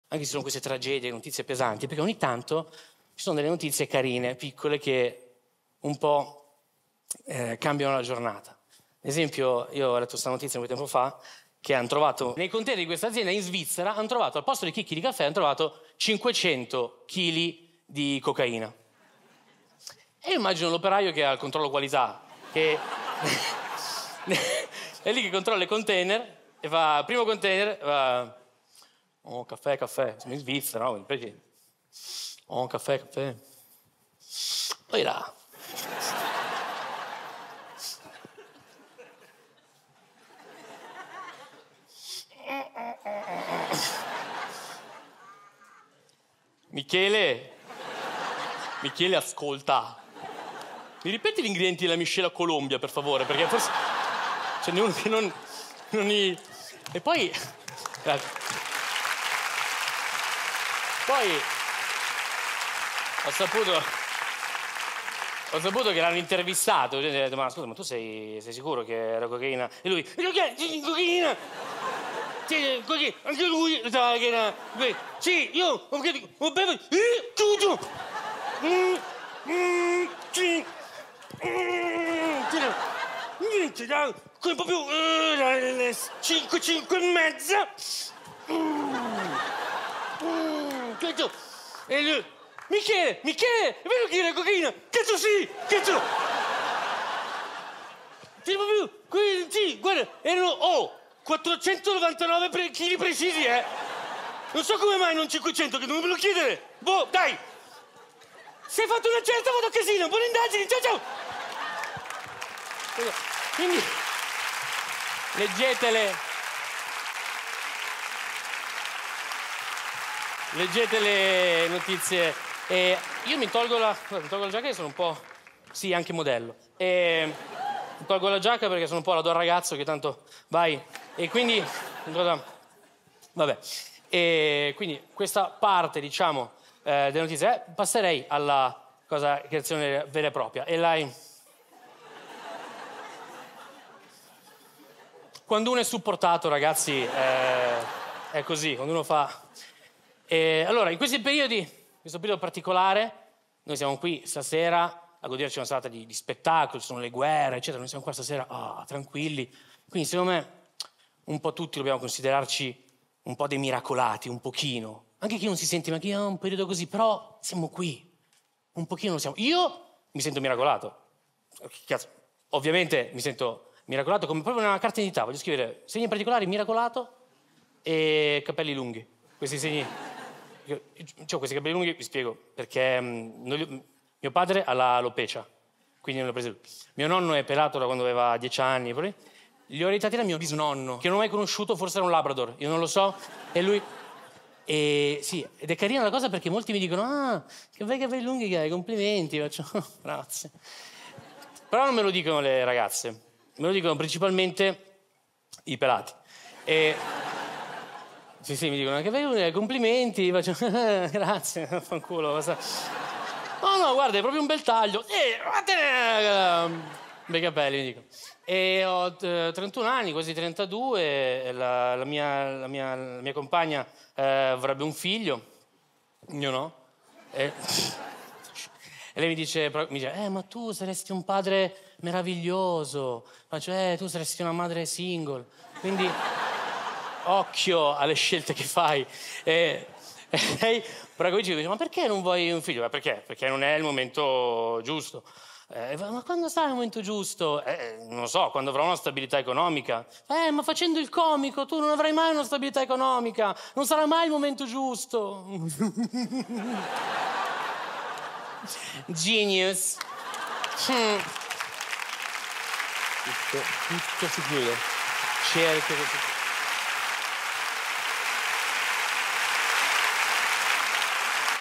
MAX ANGIONI - COCAINA, SVIZZERA E MIO NONNO
Qualche volta leggi delle notizie che ti fanno sorridere...

Tratto da "Miracolato"
Registrato al Teatro Manzoni di Milano
Montato da Federico Melzi